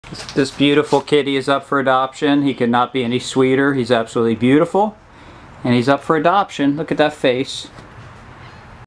Video of adoptable pet named bandito
bandito is an adoptable pet with Guardian Angel Dog Rescue in Tampa FL.